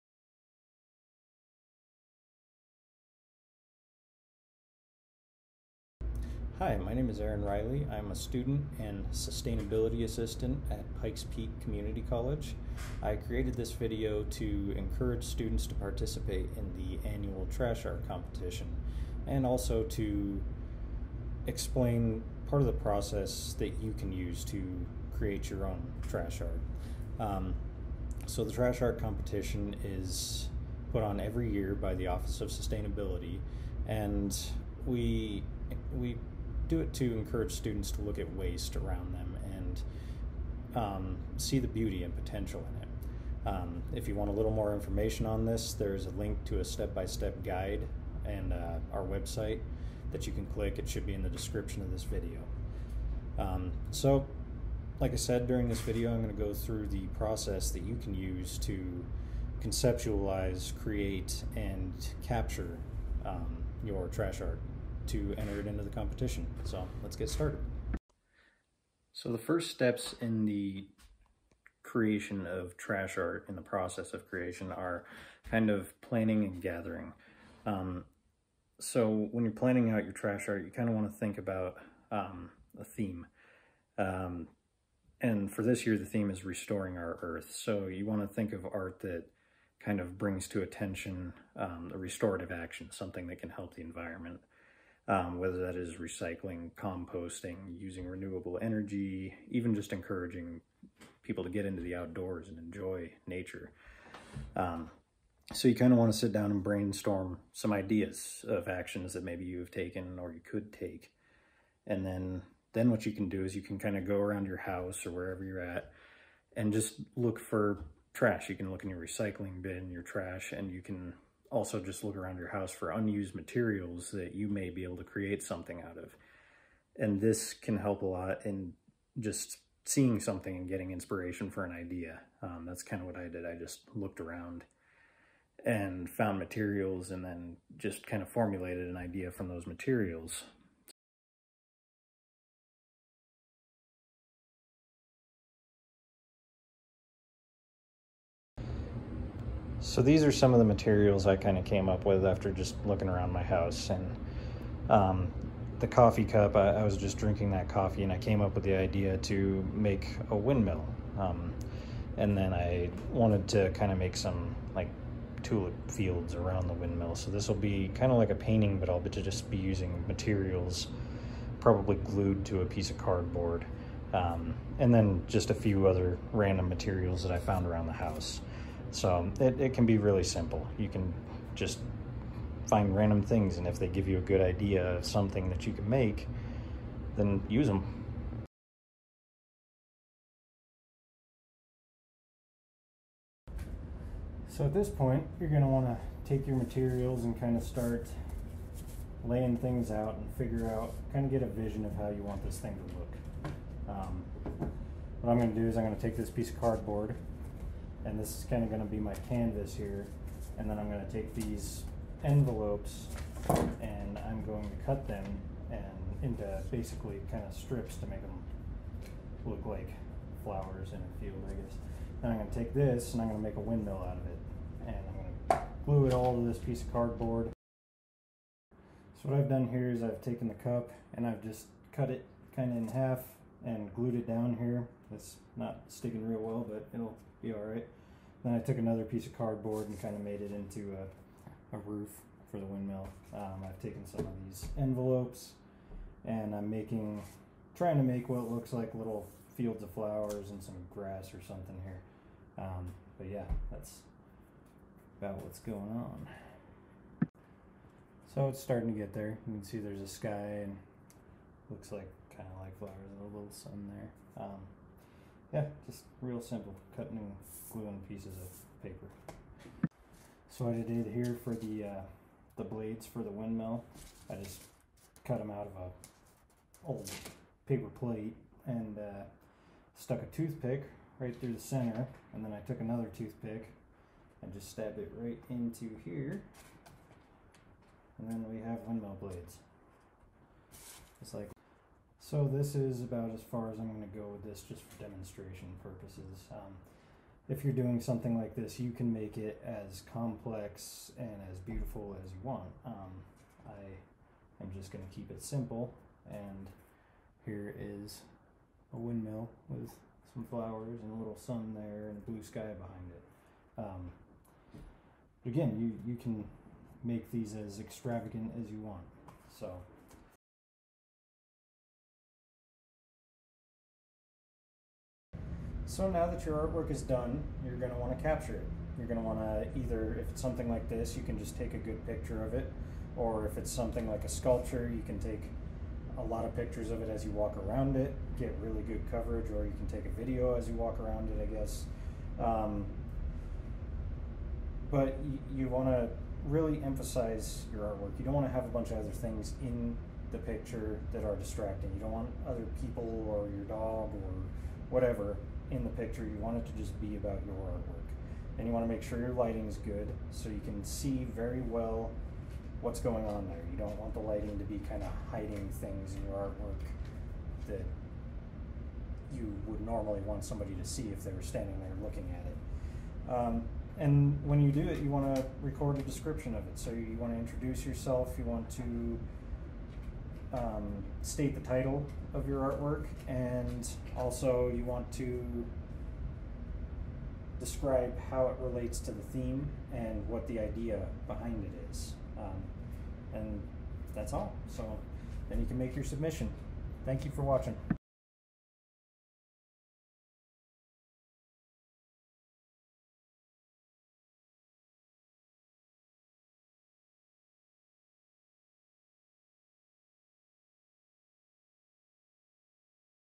Trash Art Competition 2021 DIY Video 3
Aaron Riley, Sustainability Assistant at Pikes Peak Community College, walks students through the five steps for creating trash art for the 2021 Trash Art Competition. The five steps are Plan, Gather, Create, Capture, and Share. You can find these steps, submission guidelines, and waiver form on the Office of Sustainability's